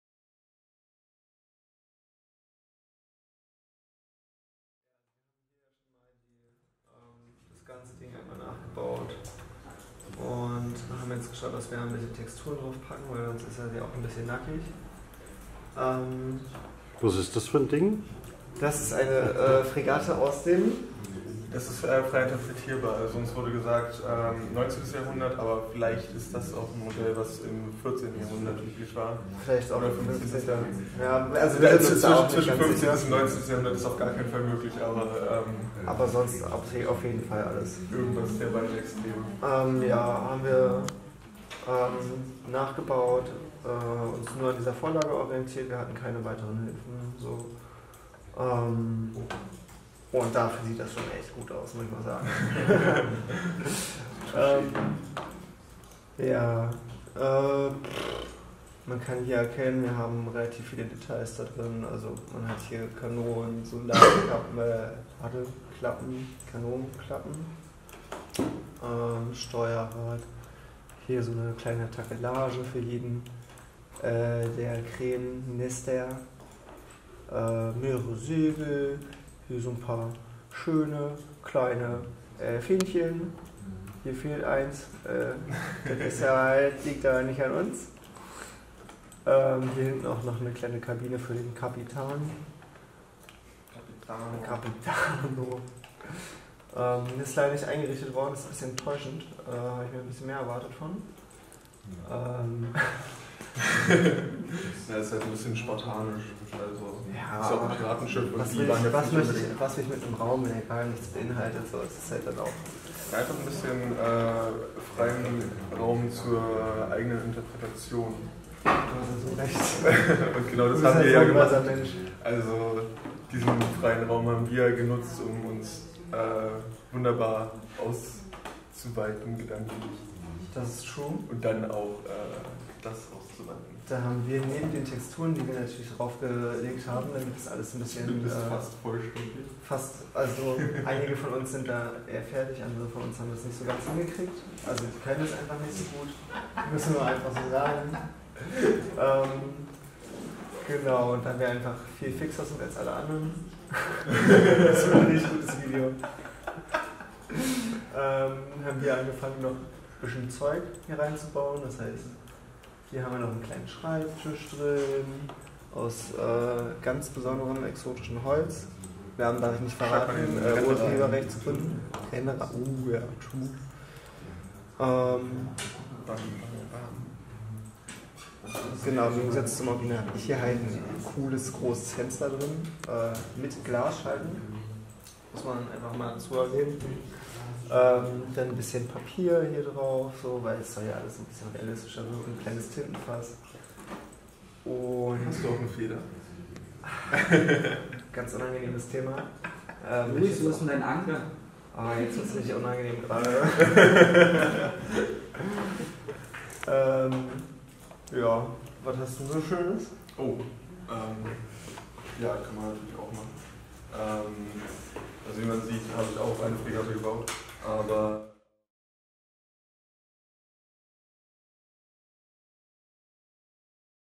inside| mediencollege berlin | Cinema 4D //02
Das Kursangebot im Fachbereich Mediendesign im 3. Lehrjahr am mediencollege Berlin ist einzigartig unter den Berufsfachschulen. Ihr habt die Möglichkeit, euch in unterschiedlichen Fachgebieten aus dem Bereich „Design“ zu spezialisieren und euer ganz persönliches Profil zu entwickeln. In unseren Modulen erlangt ihr umfassende und tiefgreifende Fachkenntnisse, welche euch zu einem selbstbewussten und sicheren Eintritt in das Arbeitsleben verhelfen - schaut mal rein!